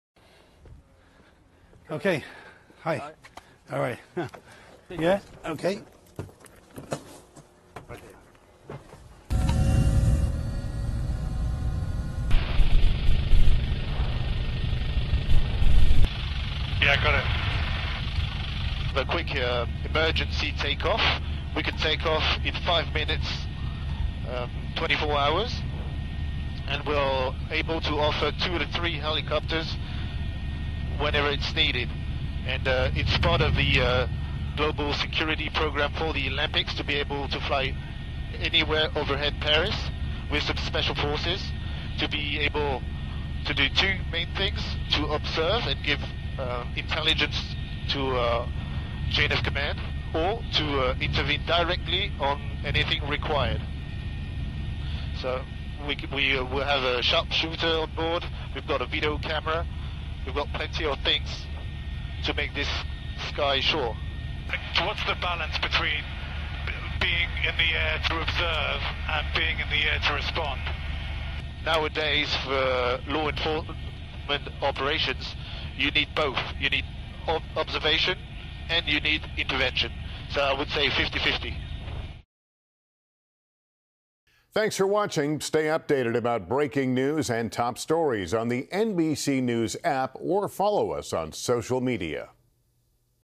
NBC News obtains an exclusive look at airborne security for the Paris Olympics
NBC News' Keir Simmons was invited on an exclusive flight with the French military police team that is providing security for the 2024 Paris Olympic Games.